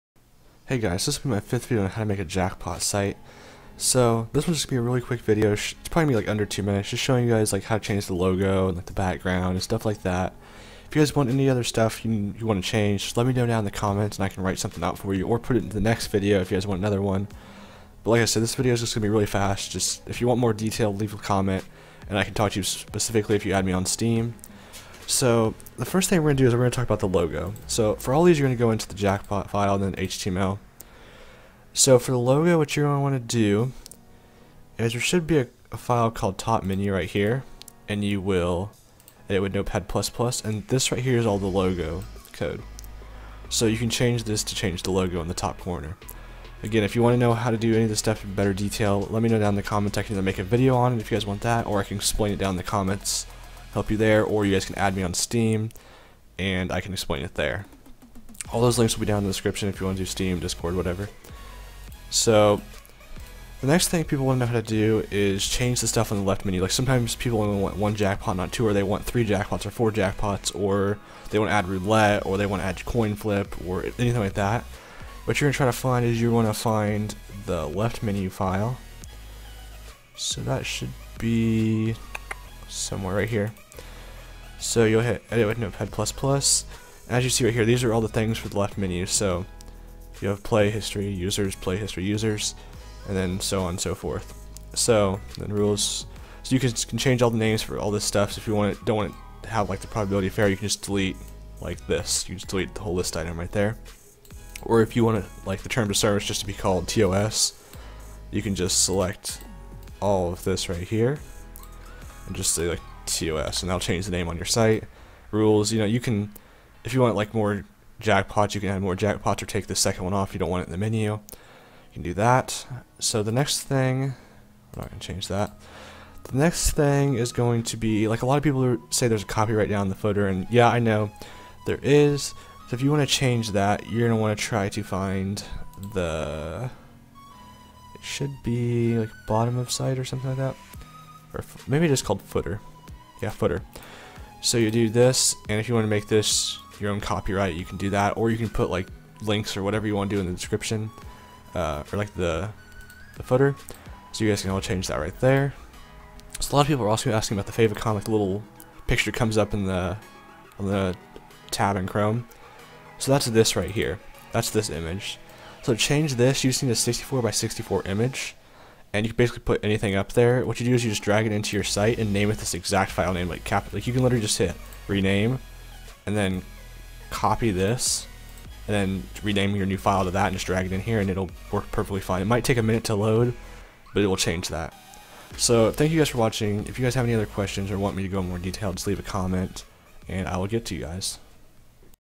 How to Make a CSGO Jackpot site #5
Thank you guys for watching my 5th video on how to make a jackpot site. In this video i show how to customize the site to how you want it, if you need more details or want to change something else let me know and I can help you!